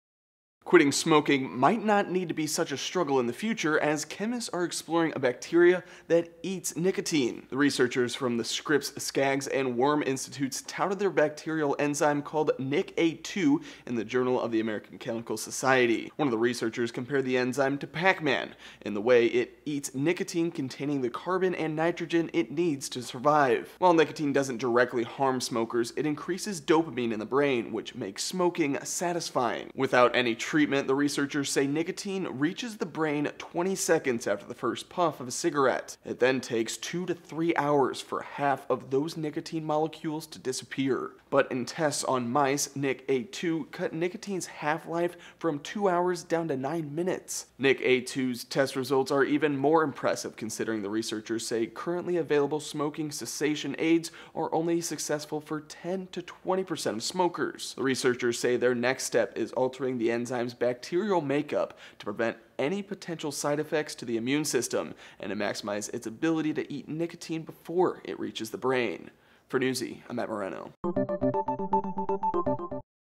A Bacteria Could Literally Eat Away Your Smoking Habit - Newsy
Researchers have found a bacteria that eats nicotine, and someday, it might help alleviate that craving when you want to quit smoking.